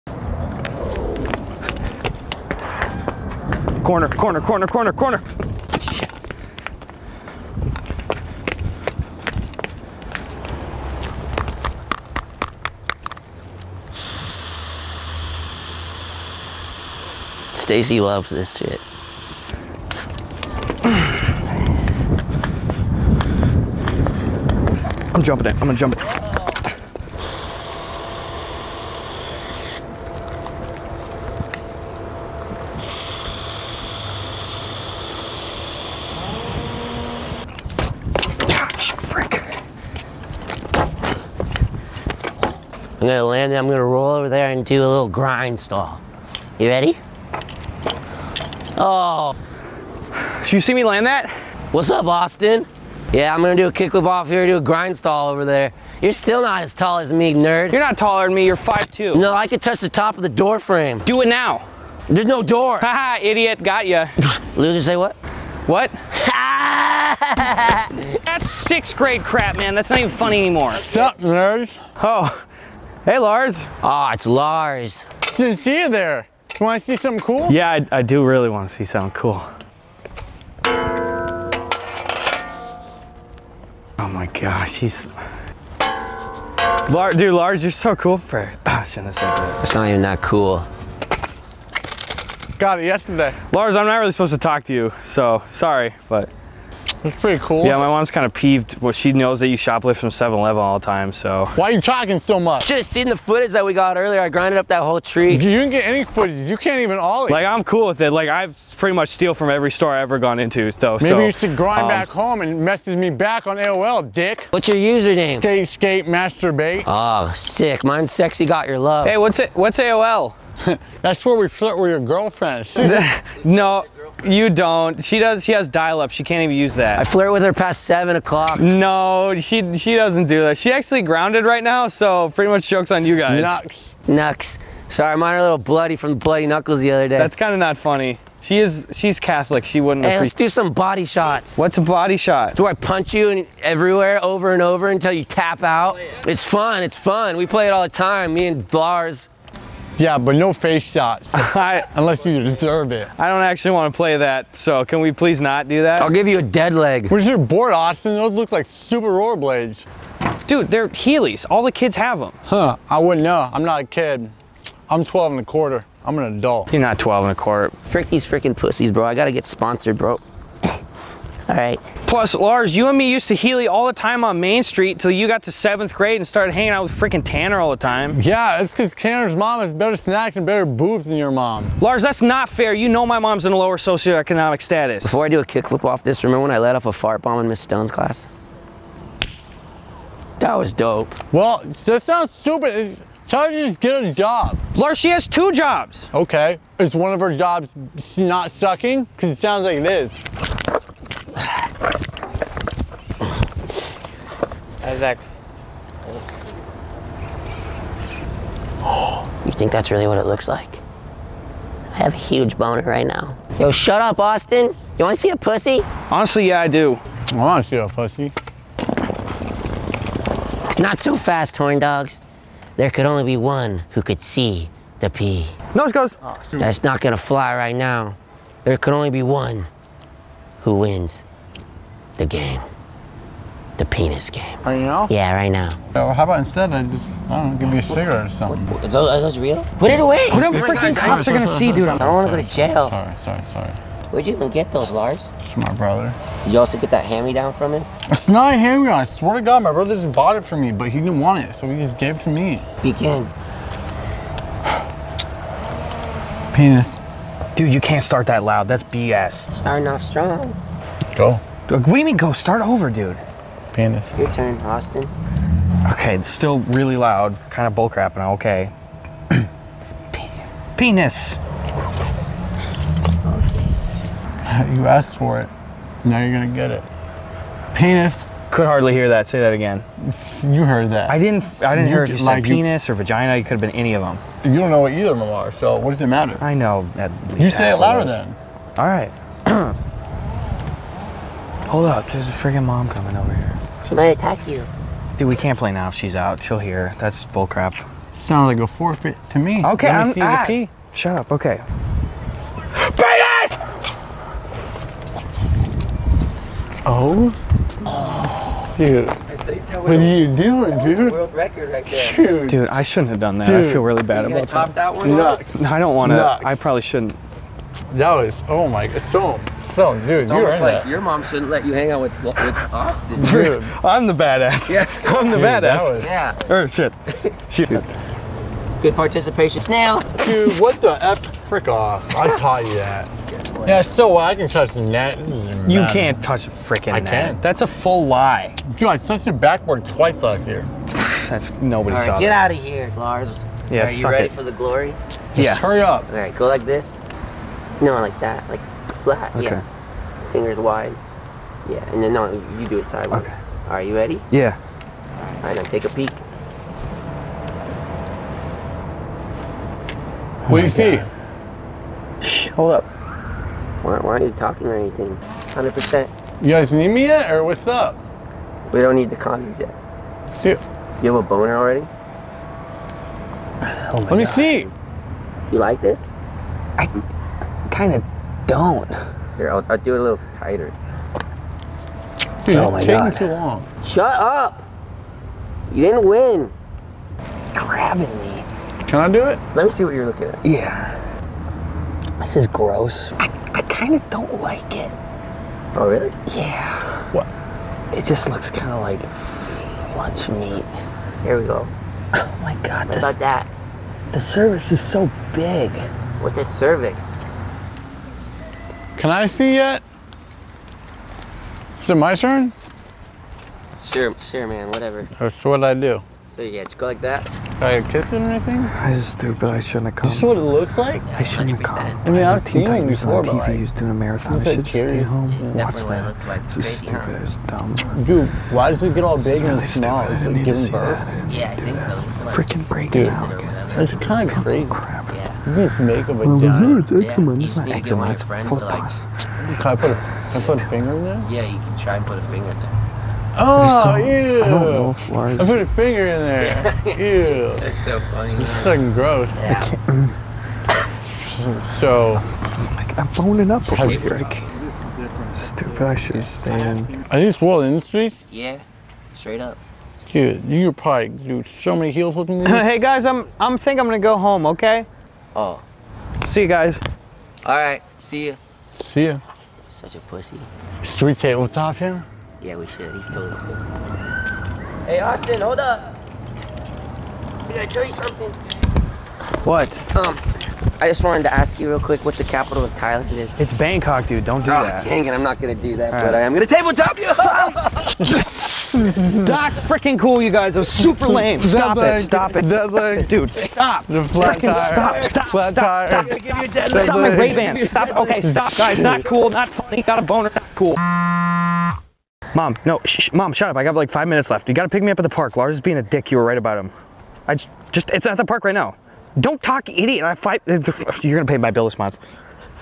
How 7th Graders Flex on Each Other
Me and my friends @TrevorWallace @gustoonz & @KevinYellow flex on each other as 7th graders